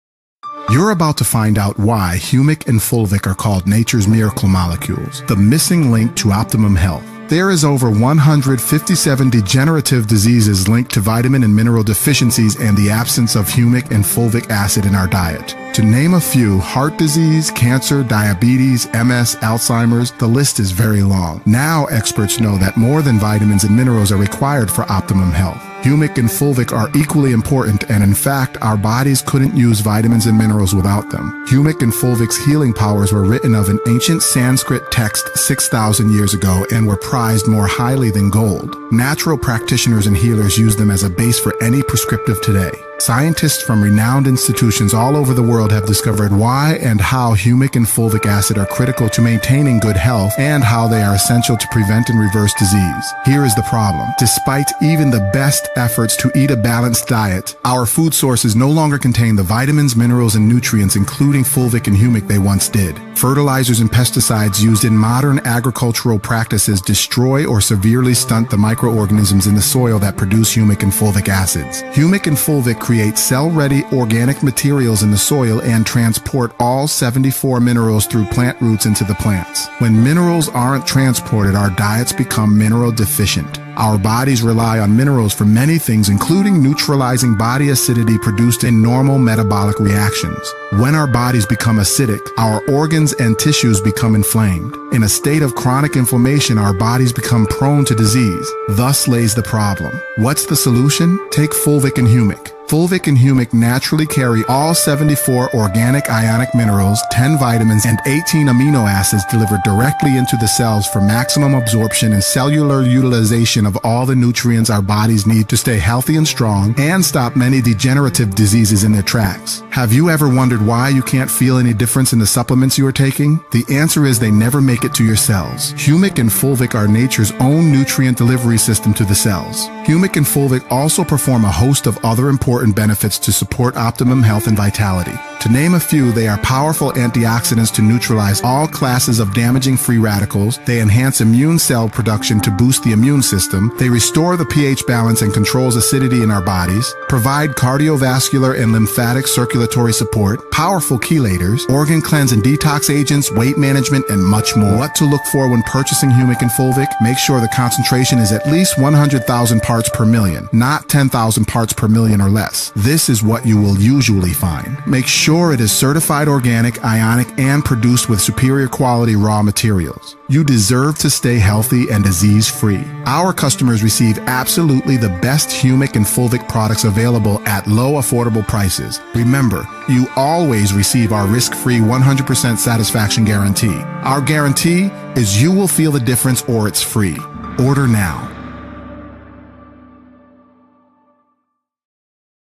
The Power of Fulvic and Humic - Nature's Miracle Molecules
Learn how Nature Nourishes plants and people.  Fulvic naturally carries 74+ Organic, Ionic "cell-ready" Minerals and Elements, along with plant-based vitamins and amino acids, and phytonutrients.  Fulvic molecules act like "Mini-batteries" charging up cells and donating electrons to supercharge cellular functions.  Potent antioxidants and free radical scavengers.  Support optimum health and wellness.